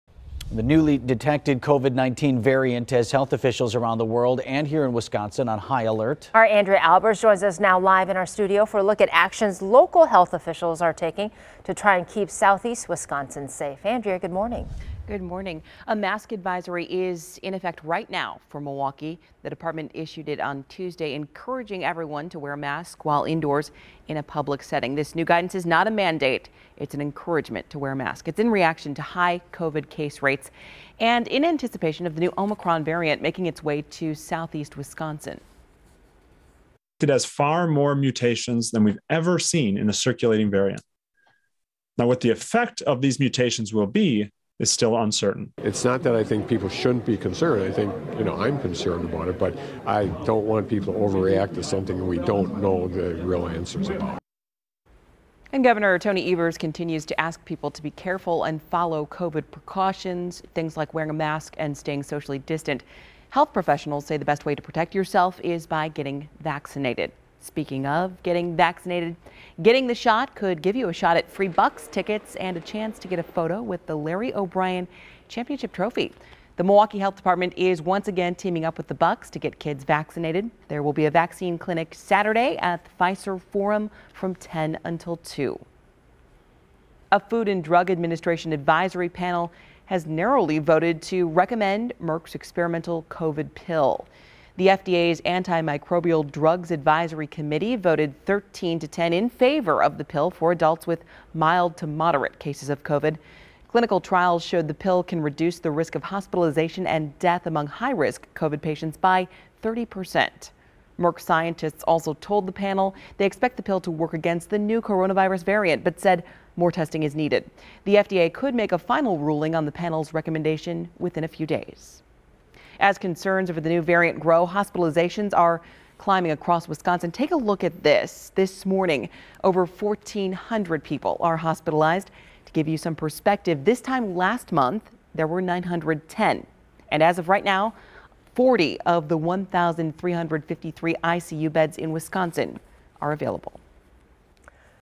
City of Milwaukee Health Department issues mask advisory due to Omicron variant
The City of Milwaukee Health Department announced Tuesday that all individuals, regardless of vaccination status or past infection, should wear a mask when in public and indoor settings due to the new Omicron variant.